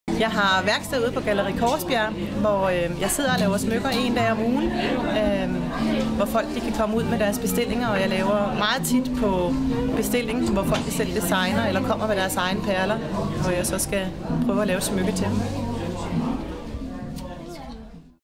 Anita Pilegaard
Kort video om Anita Pilegaard fra Pilegaard Design